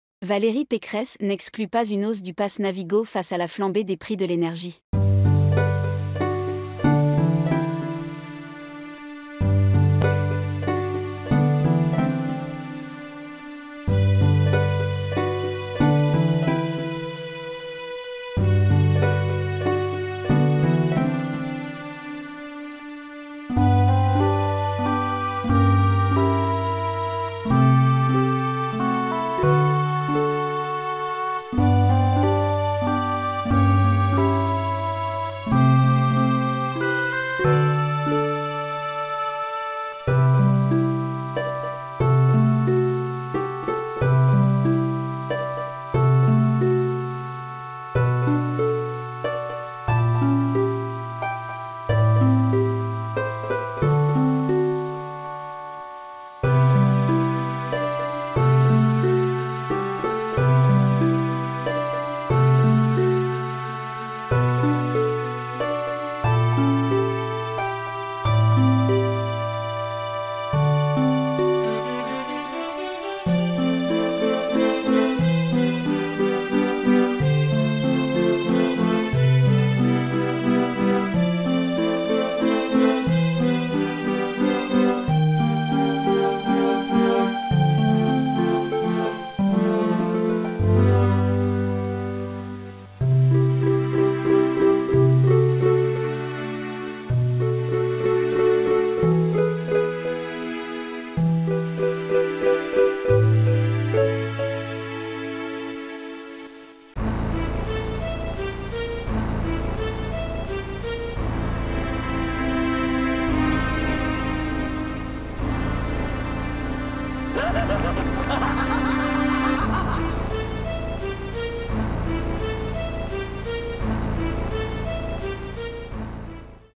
Le prix du pass Navigo va augmenter en 2023.
Valérie Pécresse n'exclut pas une hausse du pass Navigo face à la flambée des prix de l'énergie.

Coup de gueule !!! Sous le paragraphe "IMPACT POUR LES AUTOMOBILISTES ET CYCLISTES" il y a des photos révoltantes.
La honte est sur tous ces collaborateurs !!!
Et l'avis des CONtribuables ? Comme toujours, on s'en fiche.
Dans cette article, il n'est seulement mention d'une partie du tracé dans Saint-Cyr, et non de ce tronçon dans son intégralité. Évidement, sinon c'est moins vendeur.